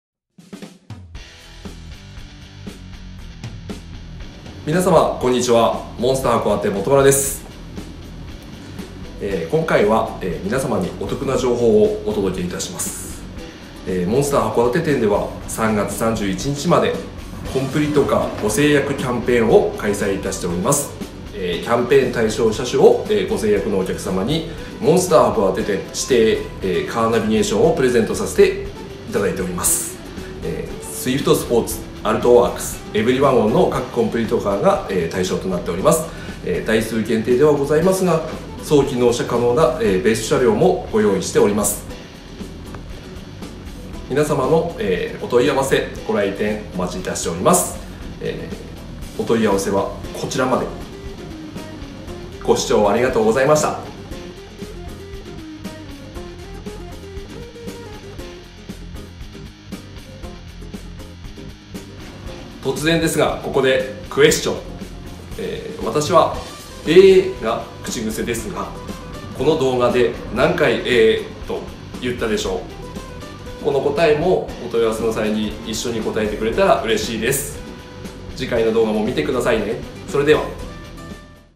コンプリートカーご成約キャンペーン 開催中 TAJIMA/モンスター函館
今回は北海道・函館のモンスタースポーツ直営店「モンスター函館」より、2020年3月開催のキャンペーンのご案内です。
※動画は2020.3.9時点の情報です。